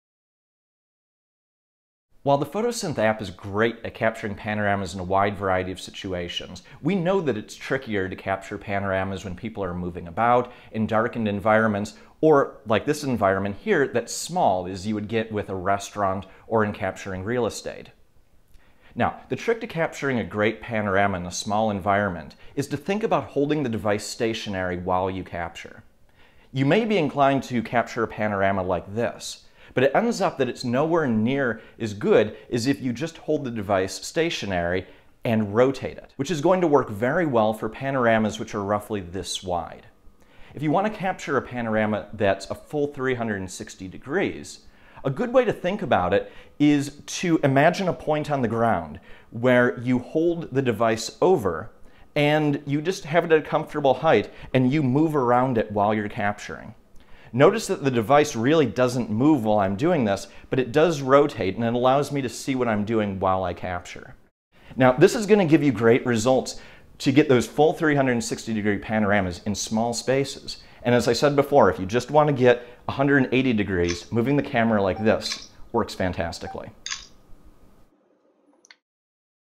Mastering the Photosynth App - Episode 1: Great Indoor Panoramas
Episode 1: Great Indoor Panoramas
In this episode, we show how to reliably make the best panoramas in small spaces with the Microsoft Photosynth App for iOS.